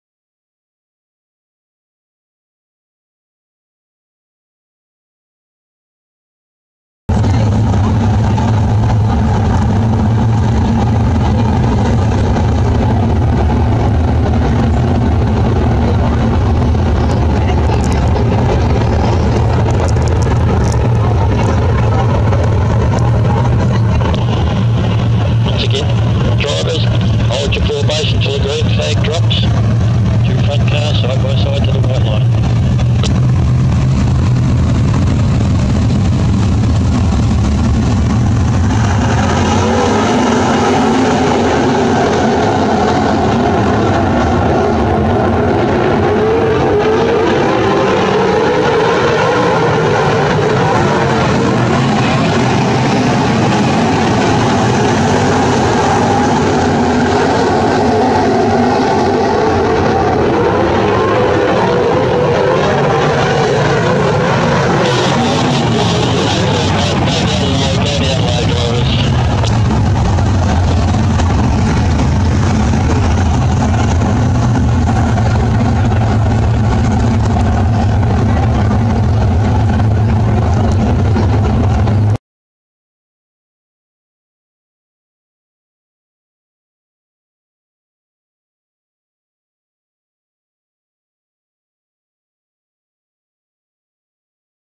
Kristy Ellis B main at Avalon until a 2nd yellow light caution 2.3.2019.
Kristy Ellis B main at Avalon until a 2nd yellow light caution 2.3.2019. The voice you can hear is the race director talking to the drivers through their one way communication.